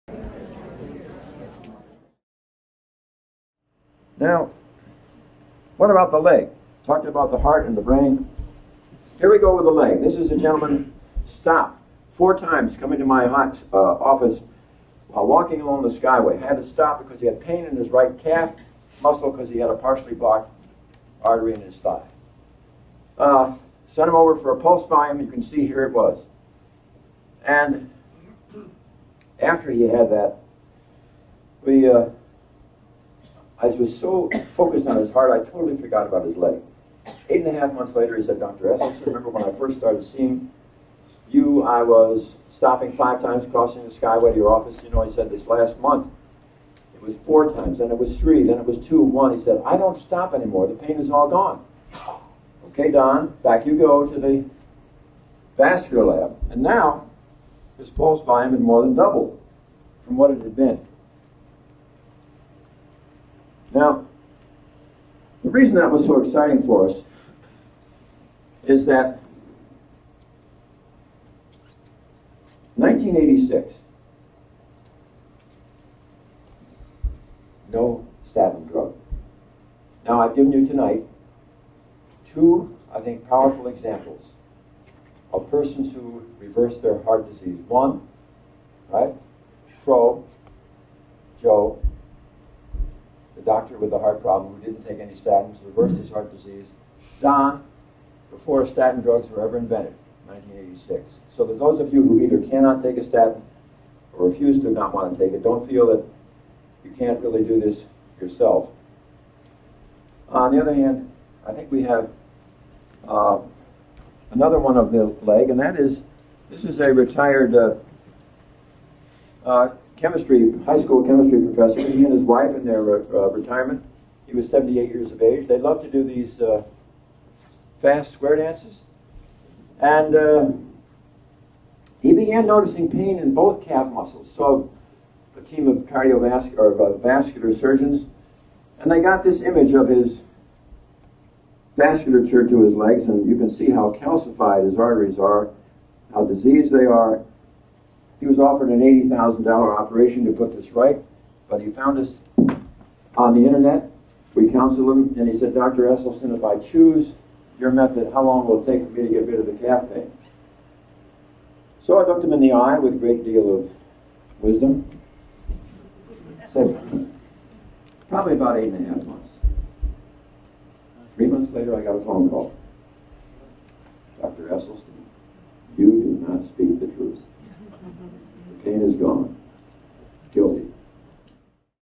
VASCULAR SYSTEM dysfunction as part of heart disease.
Treating the Cause to prevent and Reverse Heart Disease. This video discusses vascular disease. It is part of a public lecture presented by Dr. Esselstyn while visiting Sydney NS Canada in January of 2013.